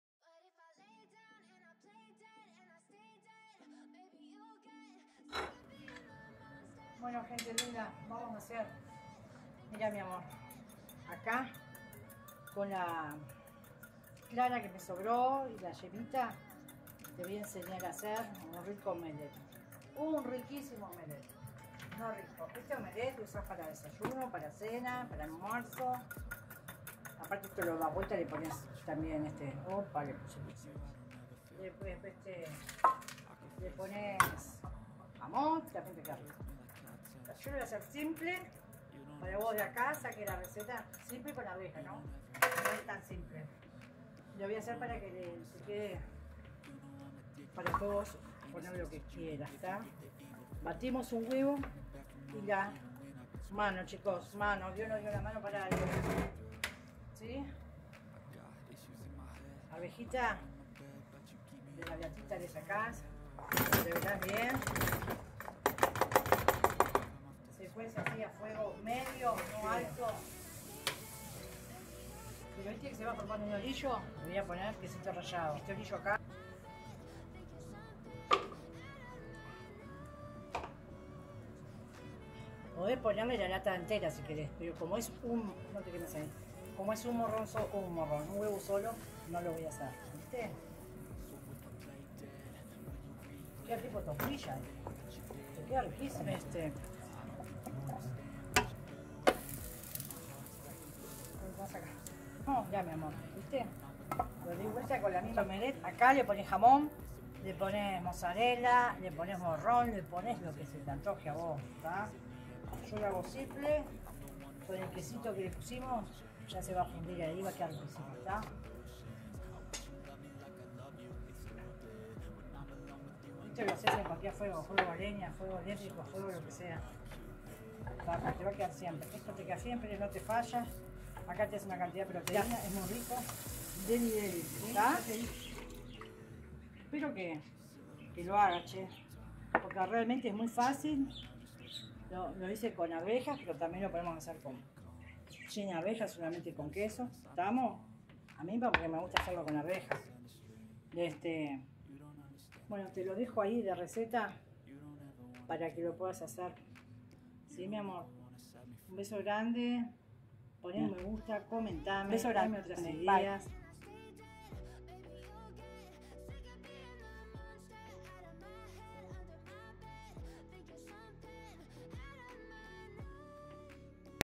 OMELETTE DE ARVEJAS MUY RAPIDO
1 CLARA
1 YEMA
ARVEJAS
QUESO RALLADO.
UN CHORRITO DE ACEITE PARA LA SARTEN.